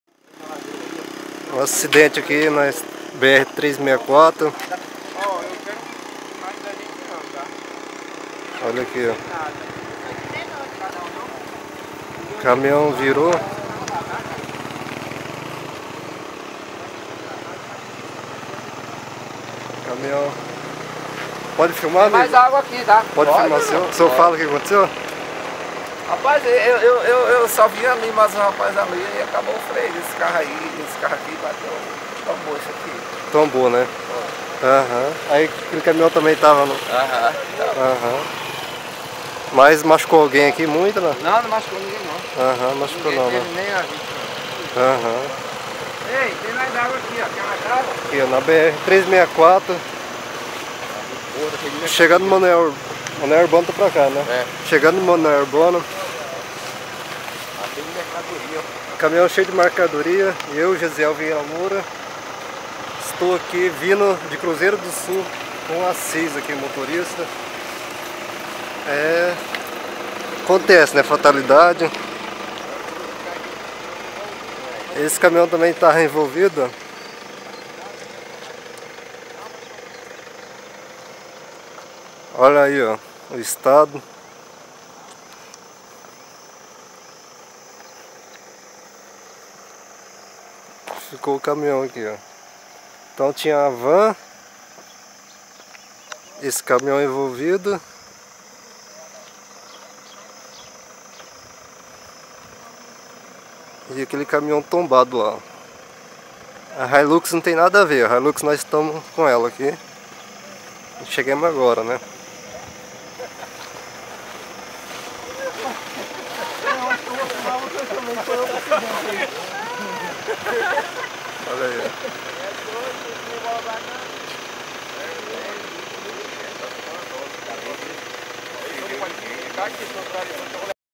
ACIDENTE COM CAMINHÃO NA BR 364 NO MUNICÍPIO DE MANOEL URBANO - ACRE
Acidente com 2 Caminhões e 1 Van na BR 364 em Manoel Urbano - Acre no Dia 19/06/2016